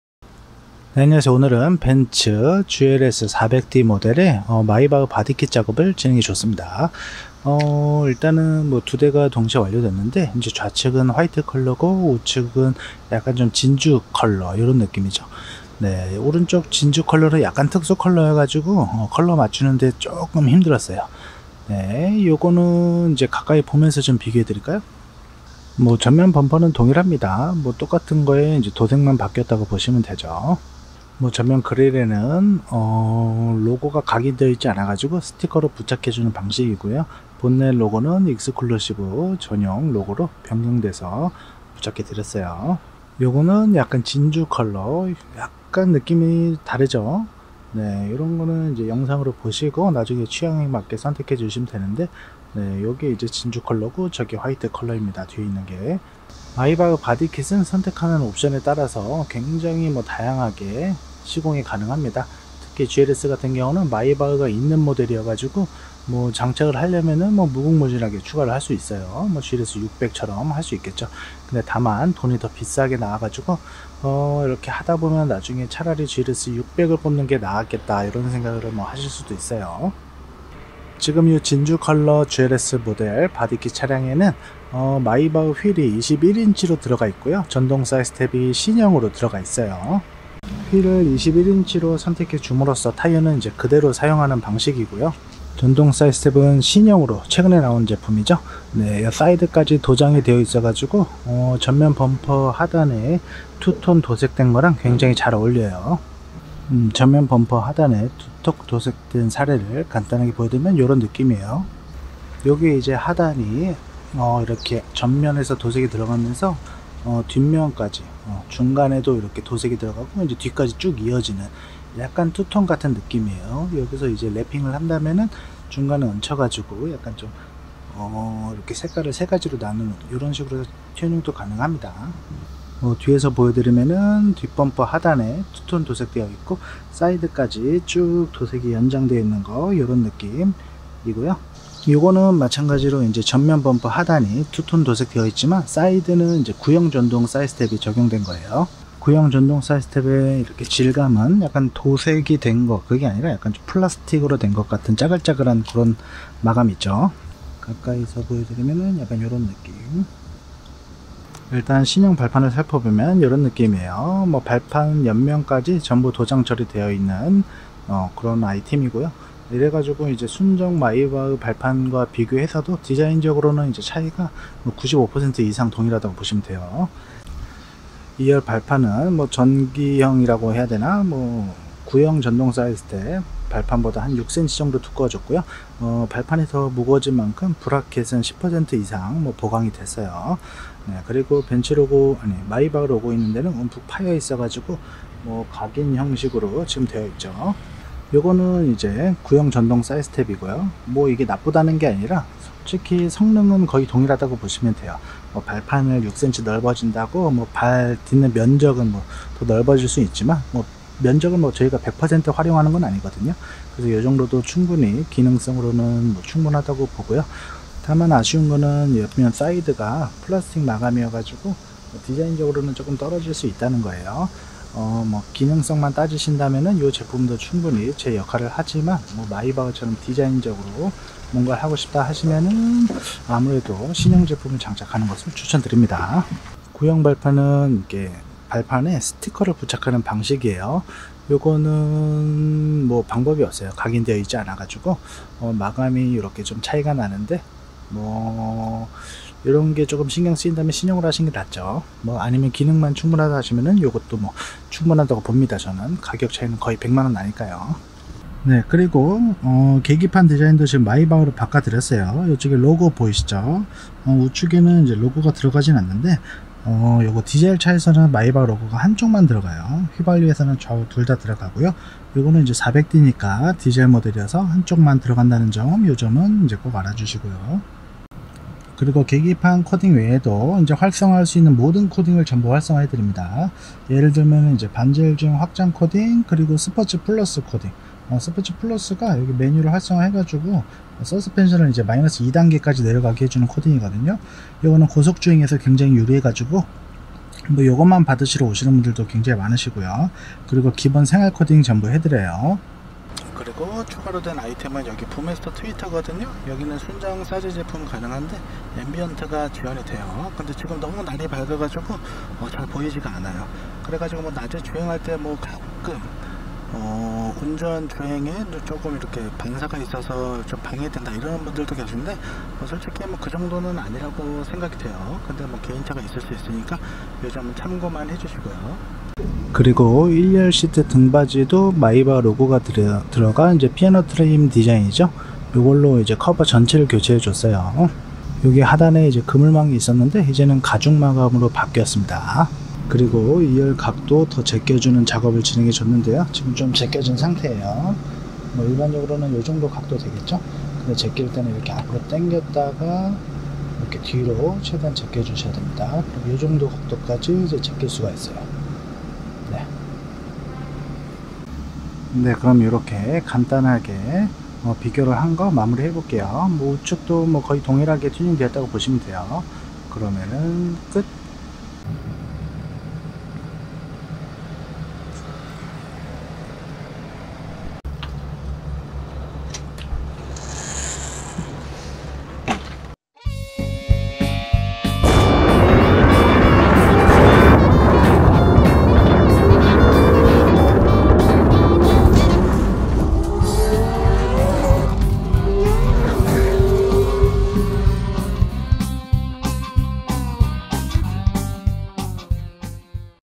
벤츠 GLS400d 마이바흐 바디킷 다이아몬드 화이트와 일반 화이트 모델 컬러감 비교해보기! GLS마이바흐바디킷
-   장착문의는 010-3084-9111 / 카카오톡 autotv / 주소 덕은동 158   -

GLS 마이바흐 바디킷 세부 아이템 설명 링크

더 많은 장착 사례가 궁금하다면 아래 링크를 참고해 주세요.